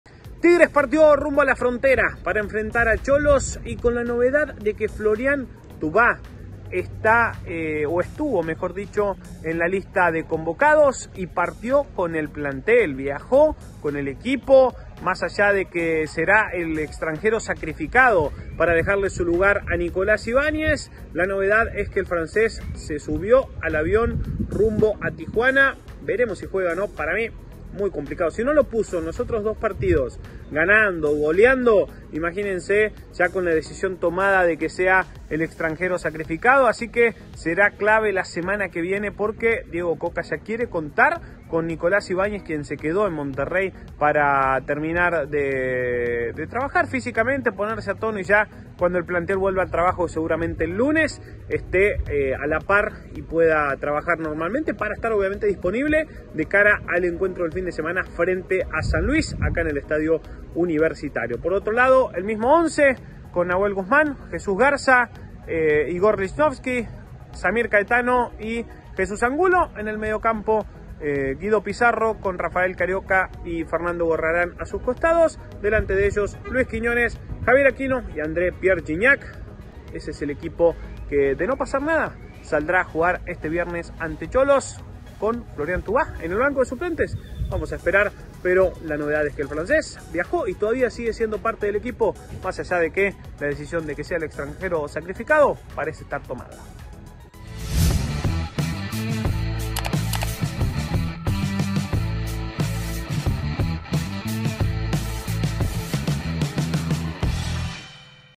¿Florian se queda o se va? Acá te traemos todos los detalles | ONCE Diario
Tigres | Los felinos partieron rumbo a Tijuana para enfrentar a Xolos, con la novedad de que Florian viajó con Tigres.